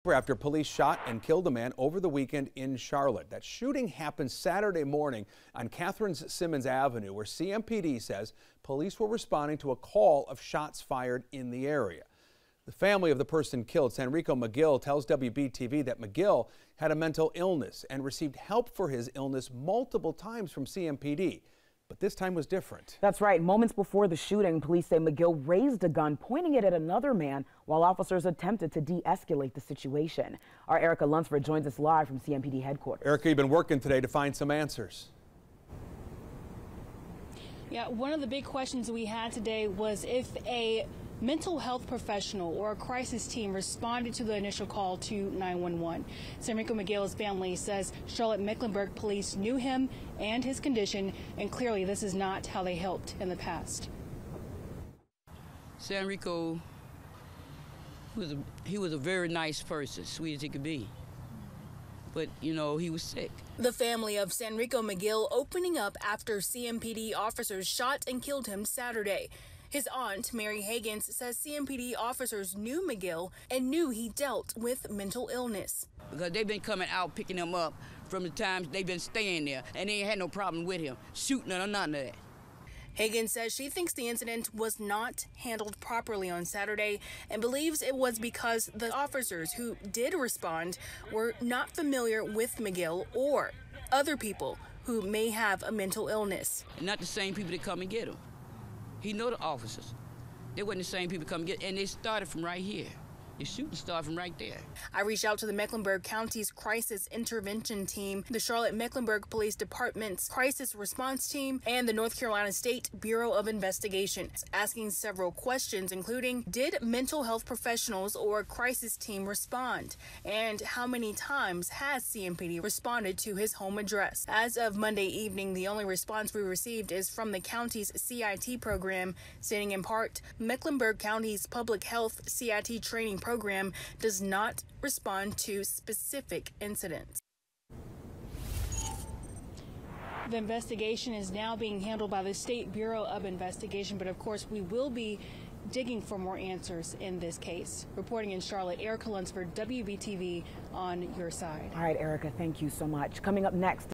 Digging Deeper: WBTV Searching For Answers After Fatal Shooting Involving CMPD Officers
WBTV searching for answers after fatal shooting involving CMPD officers